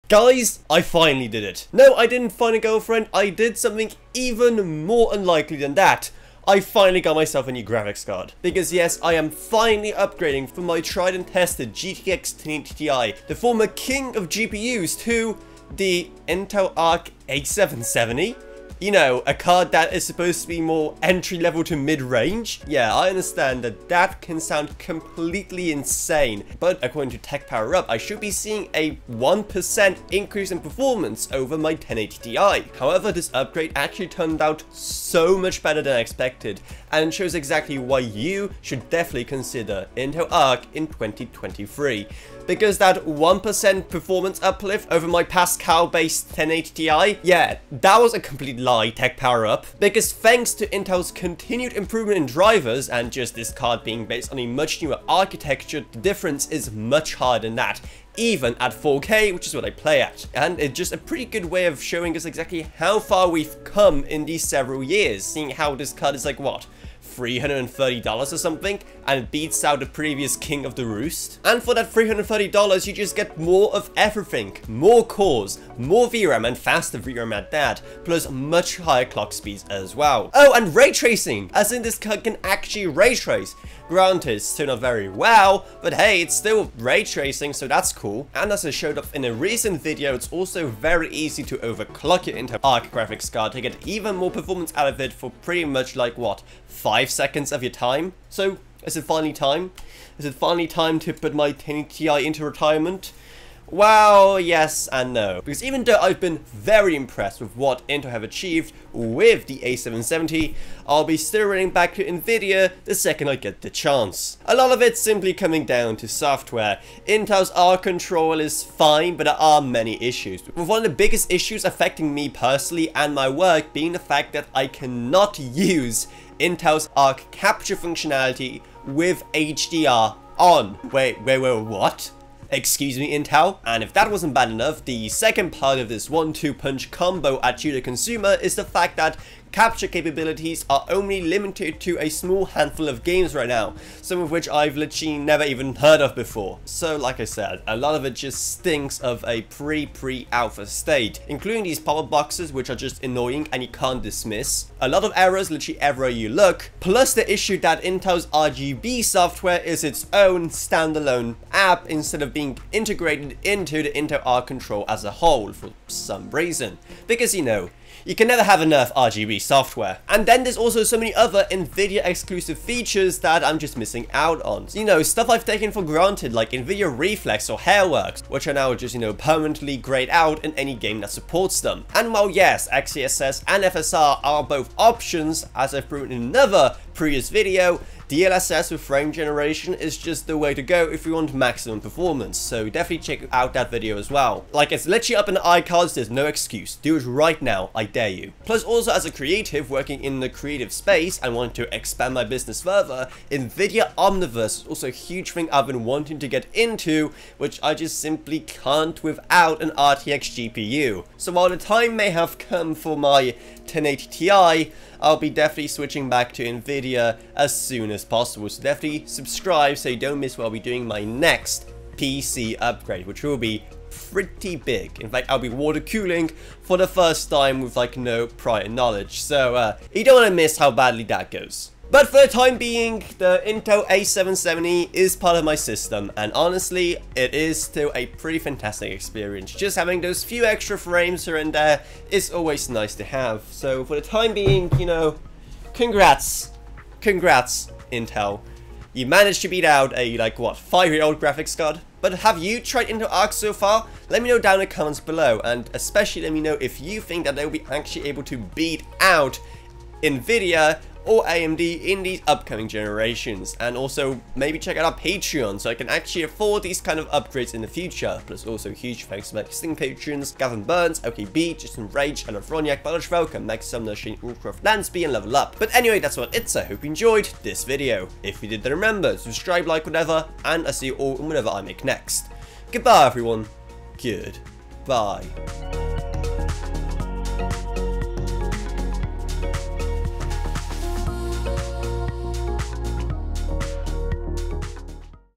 I "Upgraded" to Intel Arc?! Intel Arc A770 Performance
It finally happened, I upgraded from my old GTX 1080 Ti, to the Intel Arc A770 of all things! Why? Well because I just so happen to have it lying around. However, upgrading to Intel Arc A770 makes more sense than you think, even from such a high-end graphics card such as the GTX 1080 Ti. Nowadays, Intel Arc A770 performance gains thanks to new Intel Arc drivers are so impressive, this Intel graphics card actually BEATS OUT the GTX 1080 Ti by a nice margin!

Chapters:
0:00 Why did I "upgrade" to Intel Arc?
0:42 Intel Arc performance
1:20 Intel Arc A770 raytracing performance
1:28 Intel Arc software
2:50 What I'm missing out on